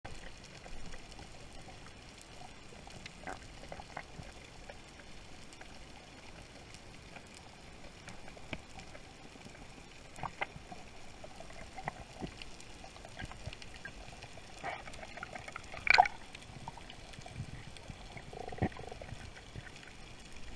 Caught This Eel PEEKING
Out freediving in Tobago and I got a nice closeup of a green moray eel hiding among the corals.